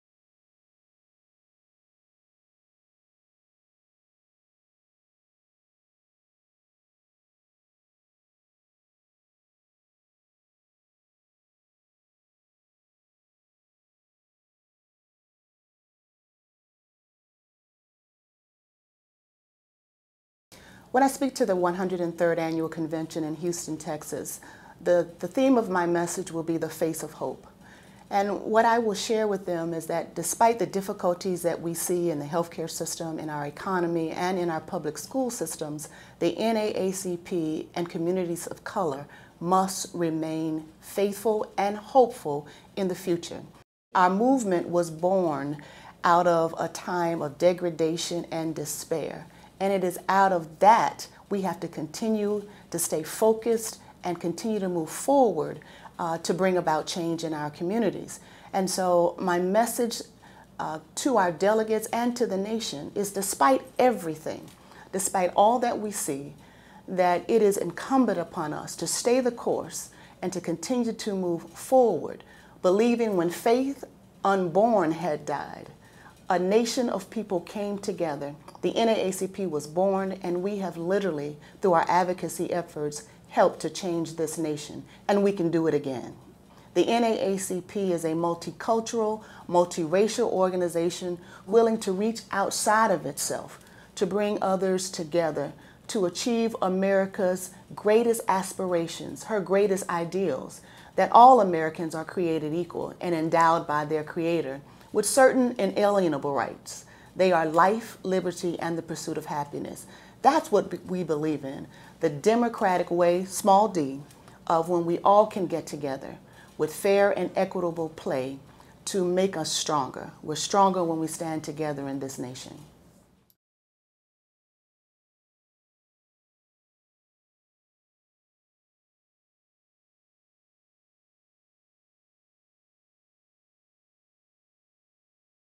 Roslyn Brock NAACP Interview 1
B.Couleur Magazine Presents: Roslyn Brock Chairman of the NAACP Board of Directors on the 2012 NAACP Annual Convention July 7-12, Houston, Texas

Roslyn discusses the theme of my message for the convention is the face of hope.